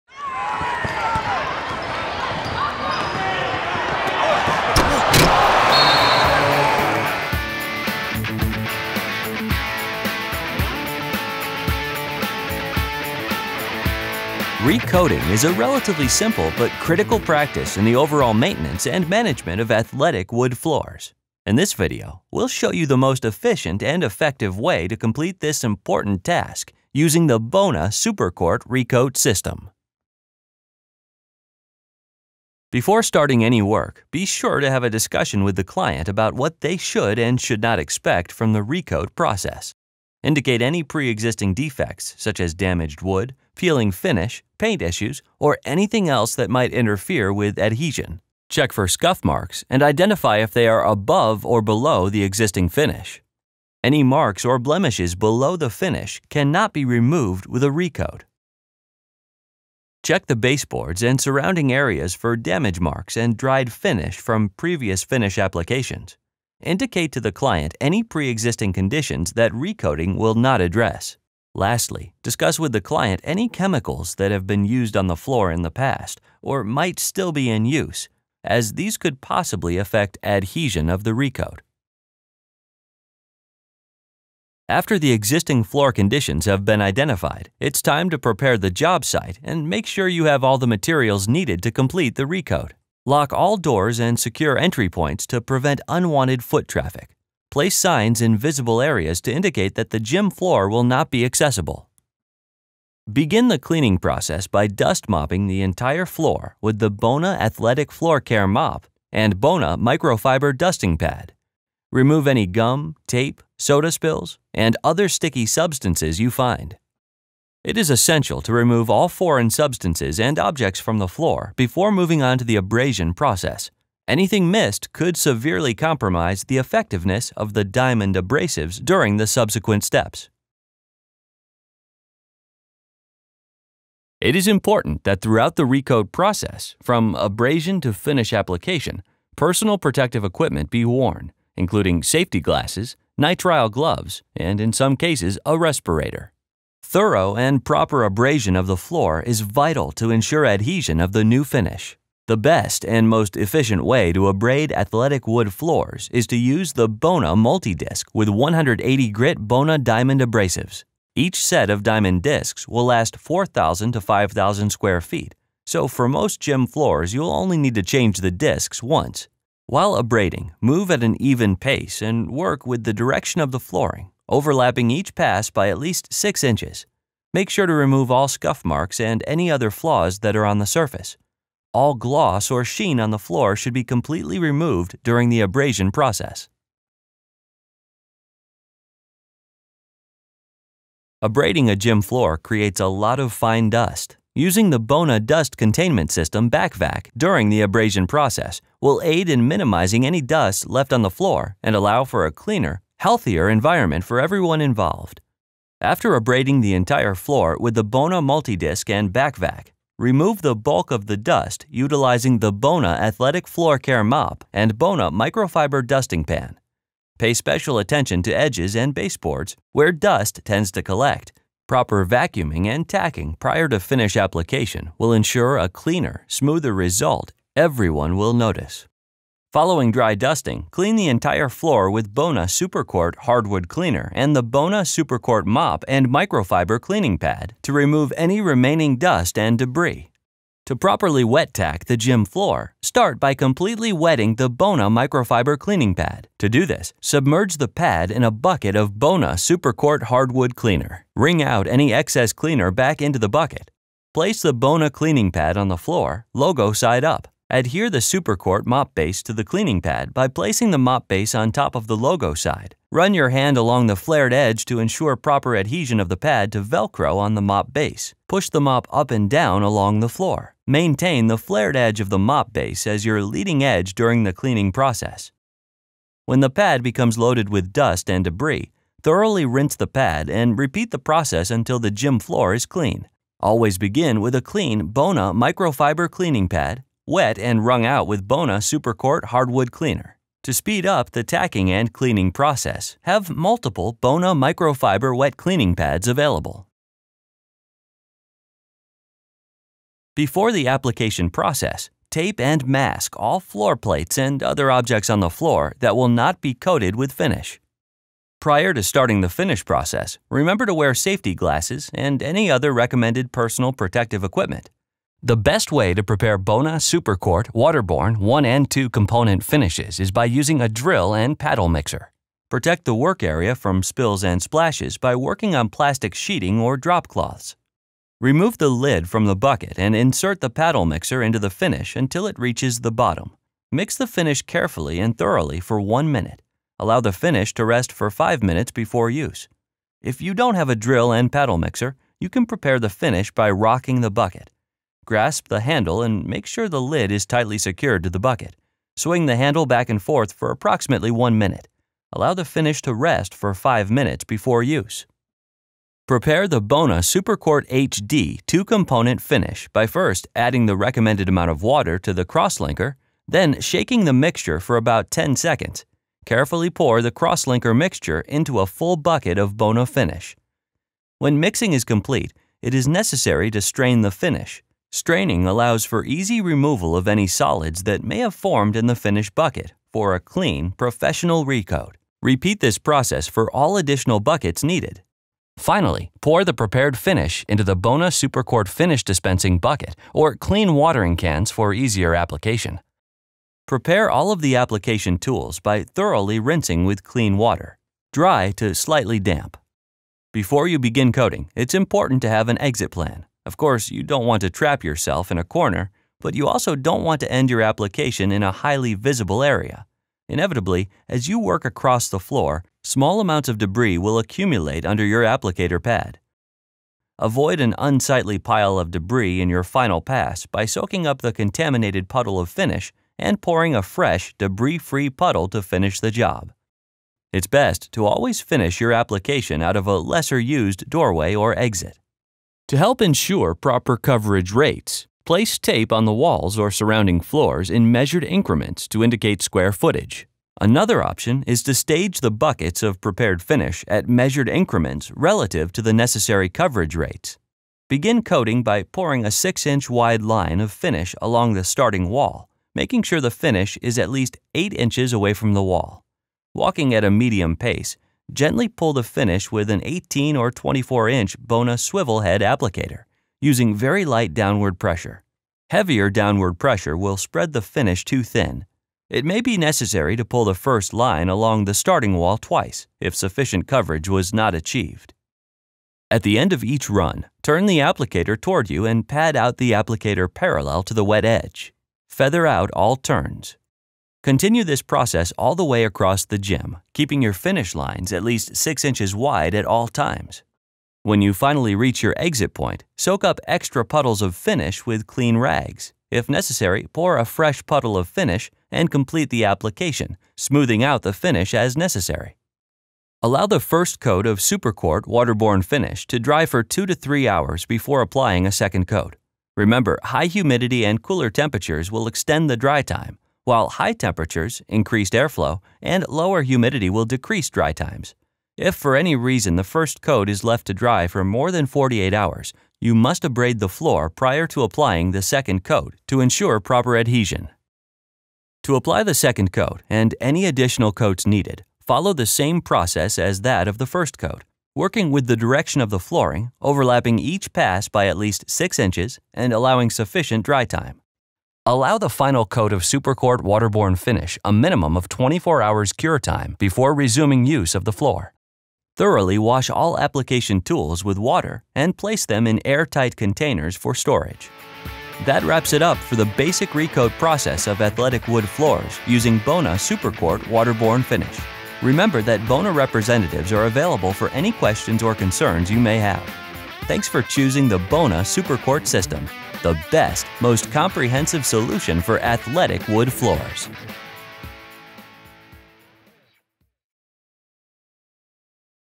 How to Recoat Wood Gym Floors with Bona SuperCourt® Waterborne Finishes
Learn how to properly re-coat athletic floors using the new SuperCourt®  Waterborne Finishes from Bona.